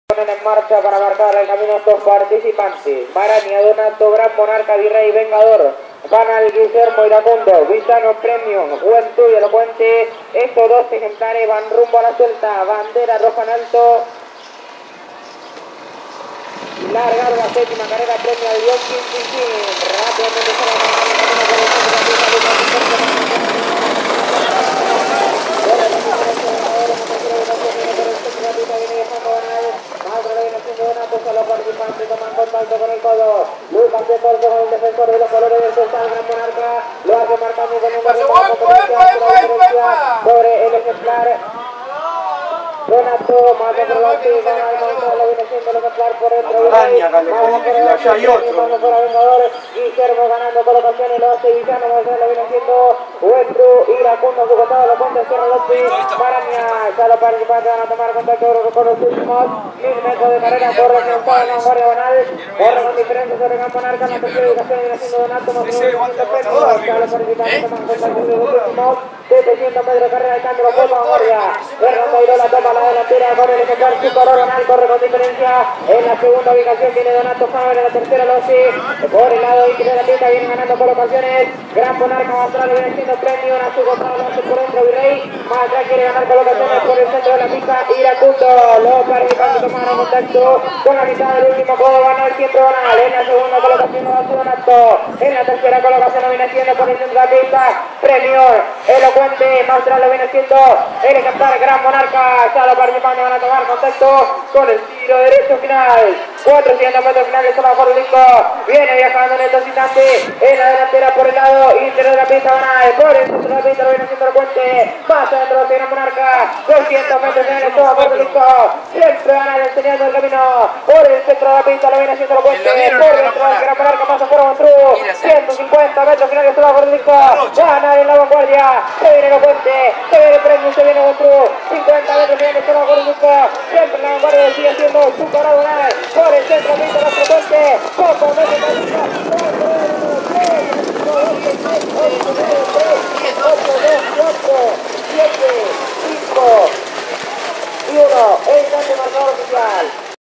7ma. Carrera. Hipódromo De Hurlingham 24 de Octubre, reunión Nº
7ma. Carrera. Hipódromo De Hurlingham 24 de Octubre, reunión Nº 38 - Ejemplar: Sis Catulo Premium - Driver: Julio Guillermo Lezama - Distancia: 1609 mts. Tiempo: 2'04''32 - Promedio: 1'17''2 pista Normal.